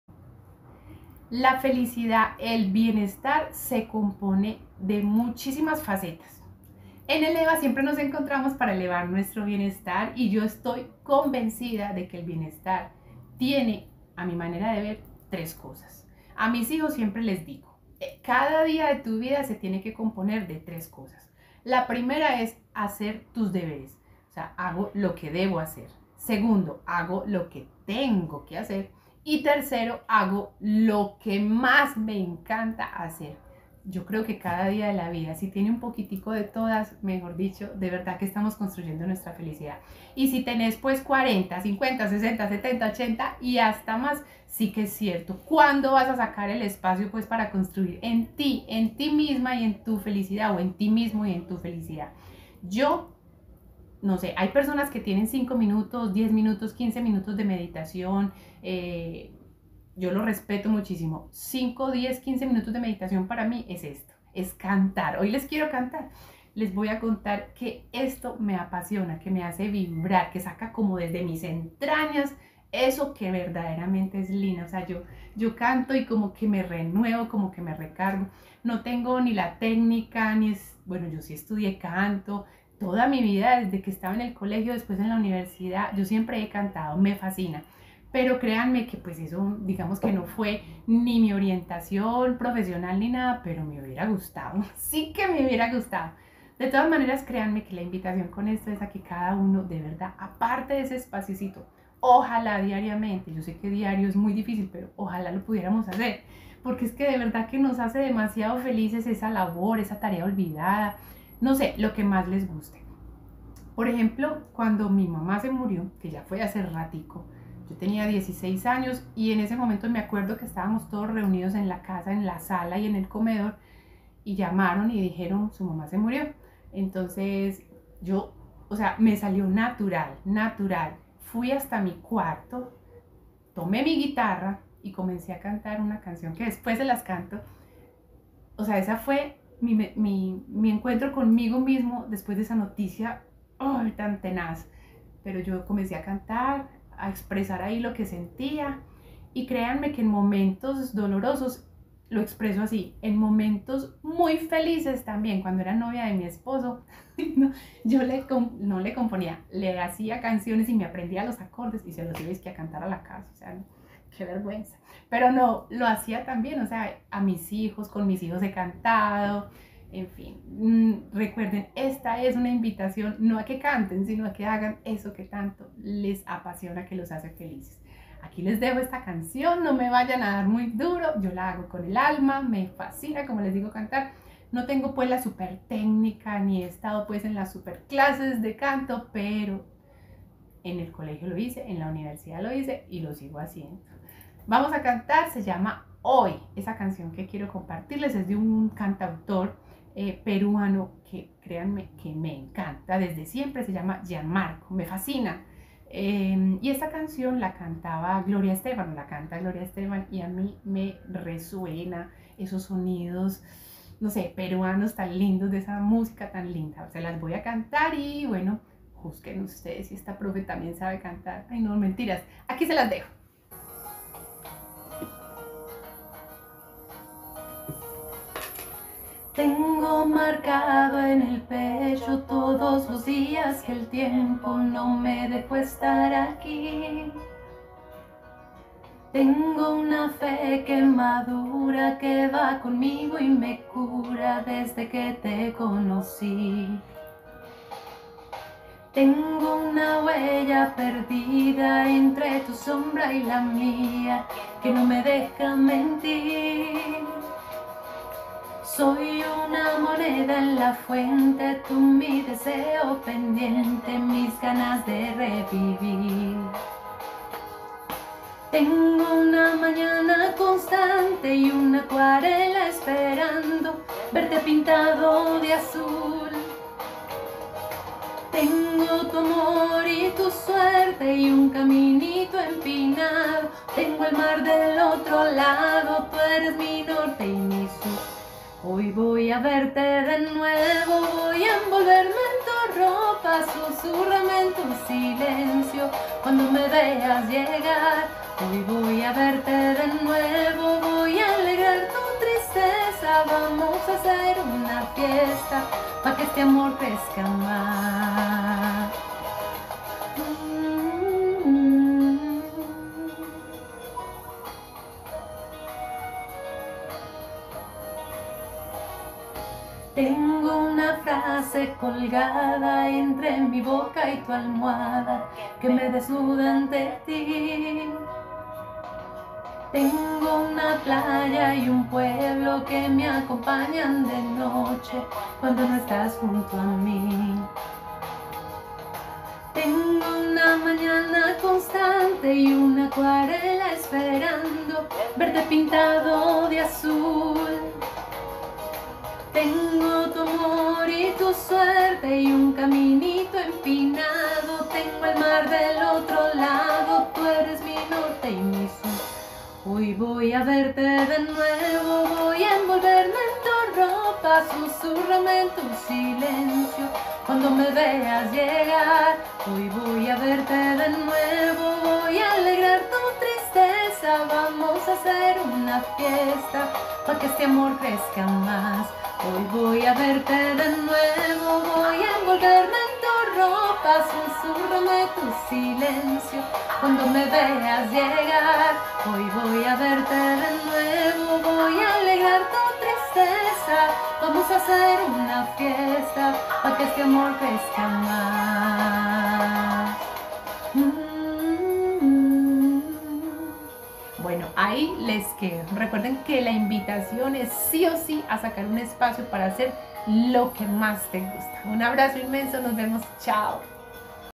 ¿Cómo construir en tu felicidad? 😄 Eleva con Lina Zuluaga
😍 El camino a la felicidad lo construimos desde nuestro interior y está en nosotros mismos 🙂 y si aplicamos esta fórmula que te proponemos, que seguramente tú también tienes para disfrutar cada día, viviremos con alegría y tendremos la suficiente fortaleza para sacar pecho ante diversas situaciones ✨.

📝💙¿Qué tal si nos compartes cómo construyes tu camino de felicidad?